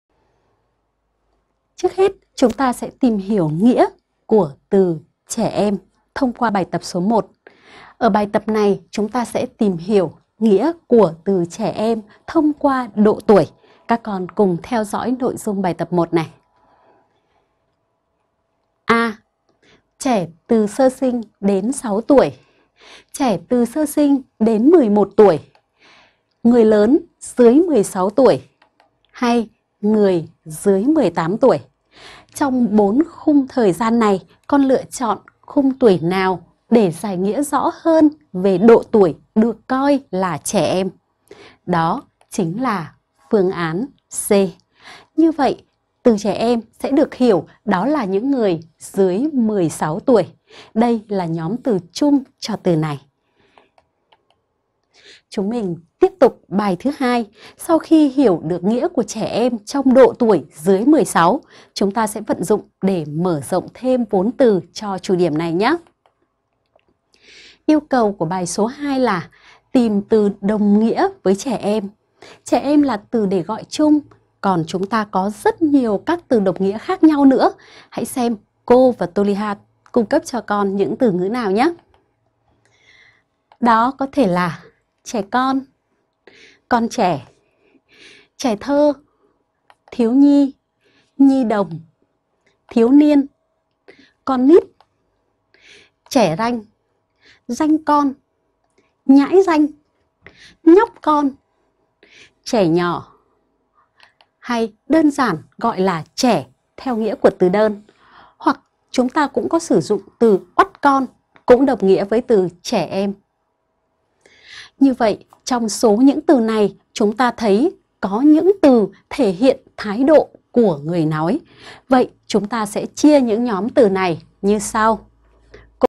Mở rộng vốn từ: Trẻ em - Luyện từ và câu - Tiếng việt 5
Bài tập 1.
Em hiểu nghĩa của từ trẻ em như thế nào? Chọn ý đúng:
a) Trẻ từ sơ sinh đến 6 tuổi
b) Trẻ từ sơ sinh đến 11 tuổi
c) Người dưới 16 tuổi
d) Người dưới 18 tuổi
Học trực tuyến bài